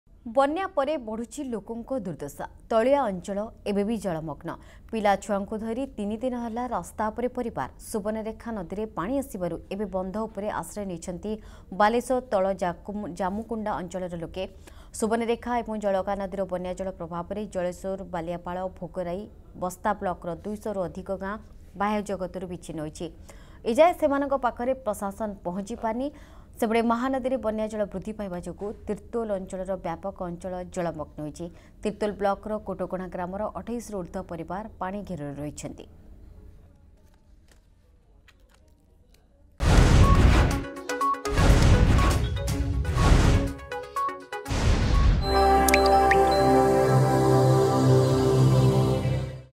ପିଲା ଛୁଆଙ୍କୁ ଧରି ରାସ୍ତା ଉପରେ ପରିବାର
ବନ୍ୟା ପରେ ବଢୁଛି ଲୋକଙ୍କ ଦୁର୍ଦ୍ଦଶା । ତଳିଆ ଅଞ୍ଚଳ ଏବେ ବି ଜଳମଗ୍ନ । ପିଲା ଛୁଆଙ୍କୁ ଧରି ତିନି ଦିନ ହେଲା ରାସ୍ତା ଉପରେ ପରିବାର ।